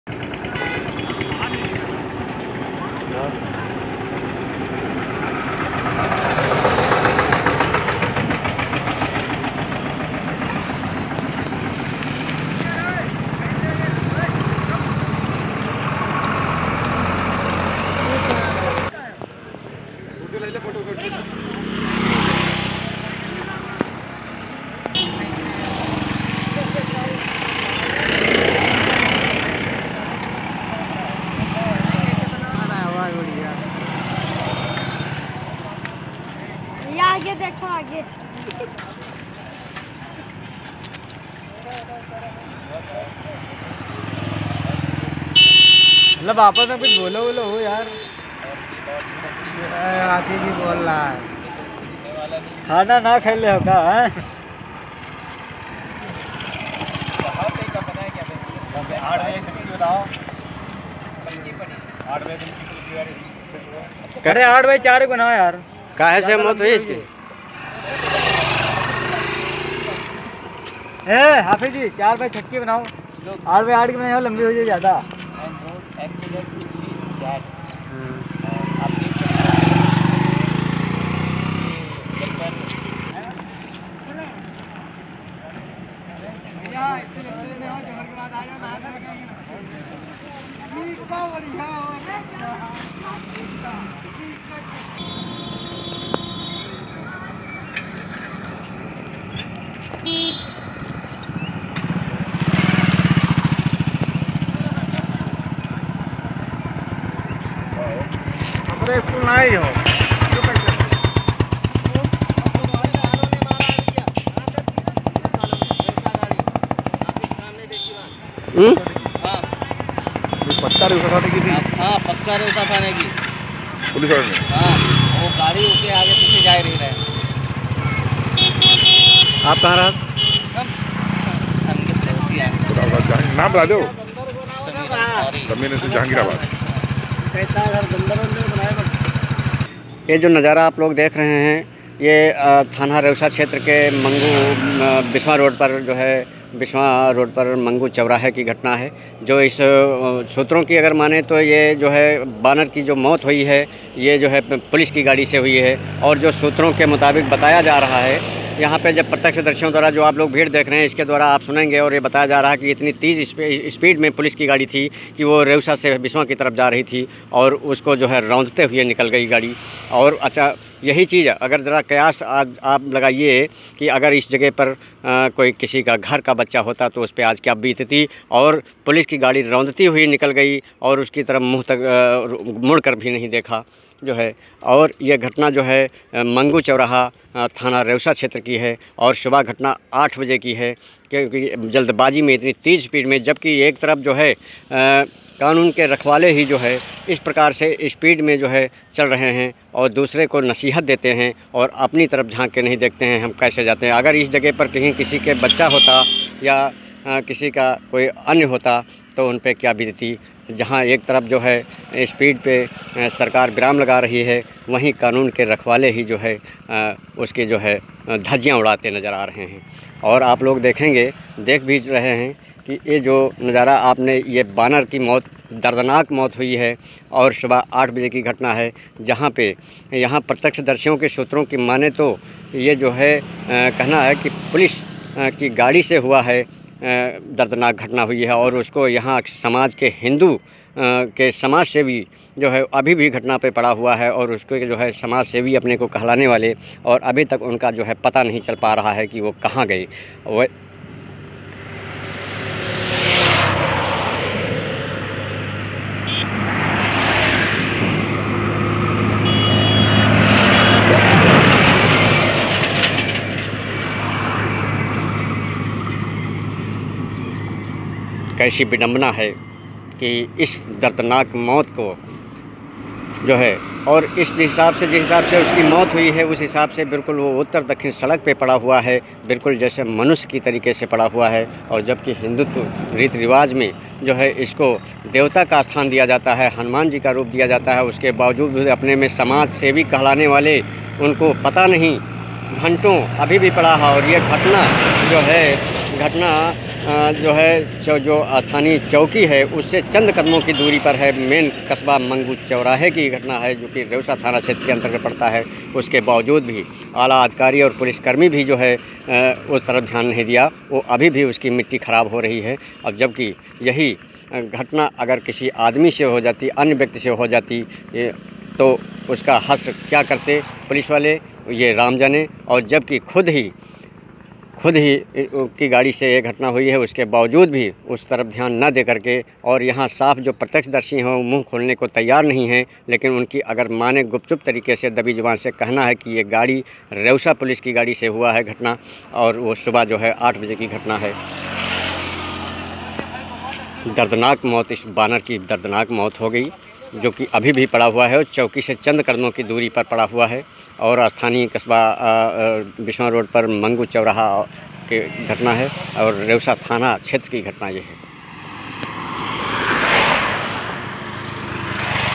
चौकी से चंद कदमो की दूरी पर।
सीतापुर-रेउसा थाना क्षेत्र में पुलिस की गाड़ी से बानर की कुचलकर मौत।चौकी से चंद कदमो की दूरी पर।